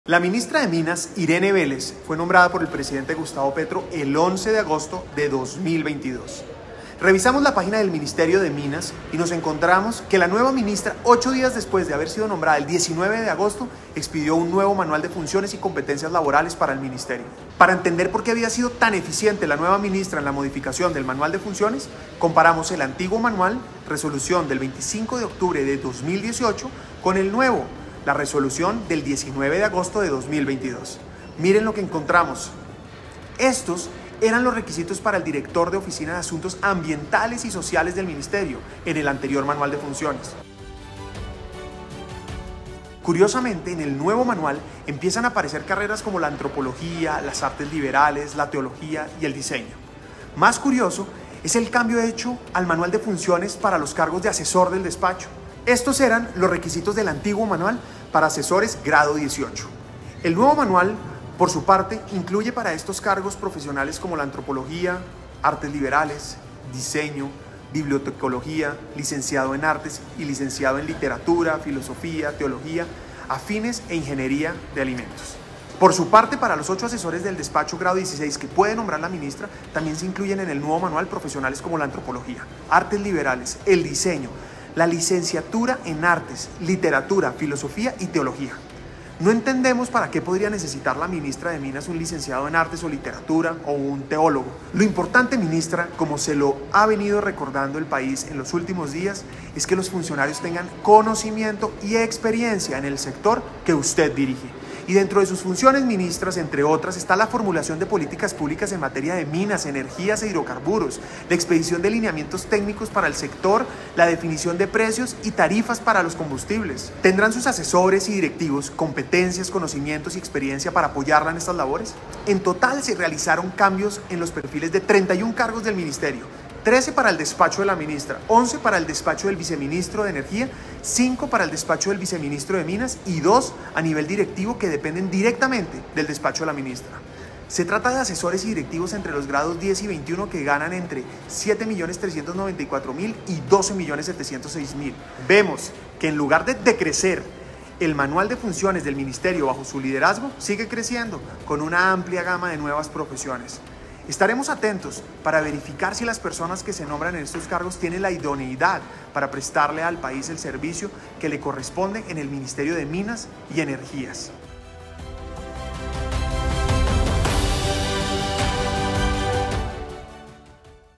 ¡Denuncia!
¿Por qué la Ministra de Minas y Energía, Irene Vélez, ha cambiado tanto el manual de funciones y competencias laborales en su ministerio?